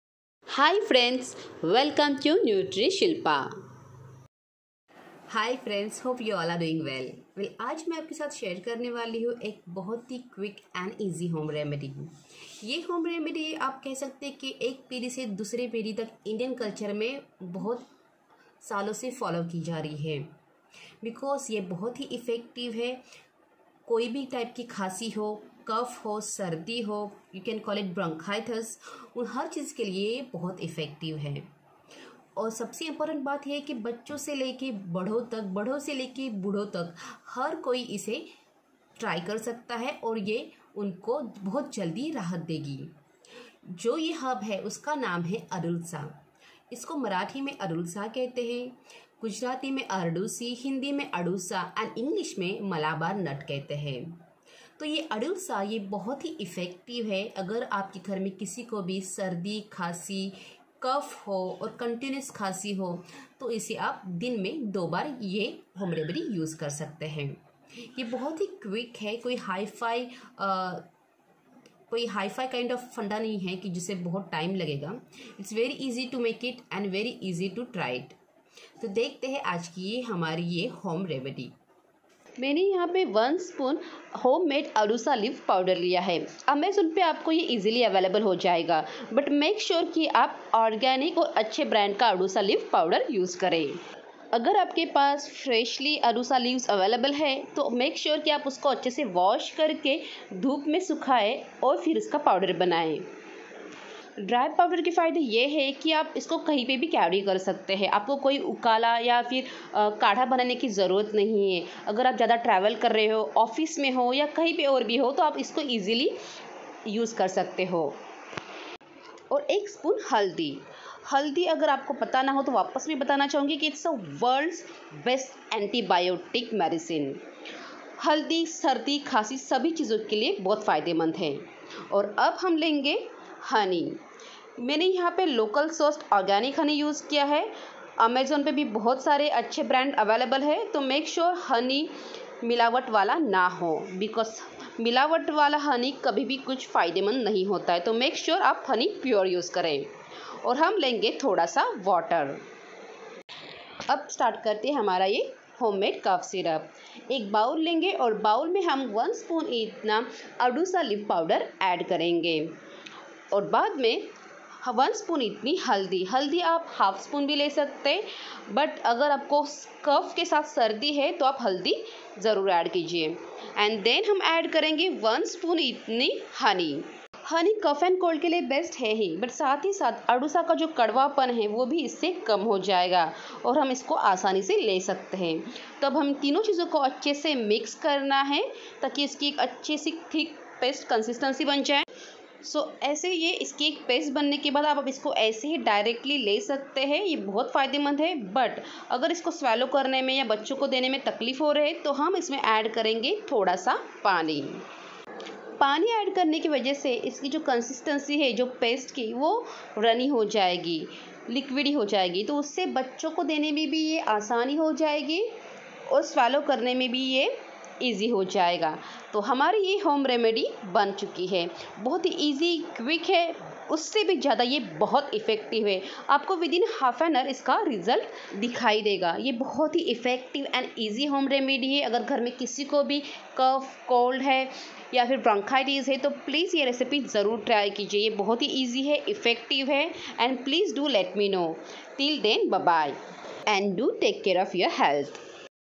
Take one spoon of this home remedy and say bye bye to cough -cold, bronchitis , asthma.
This home remedy really work for respiratory congestions like cough, bronchitis, asthma.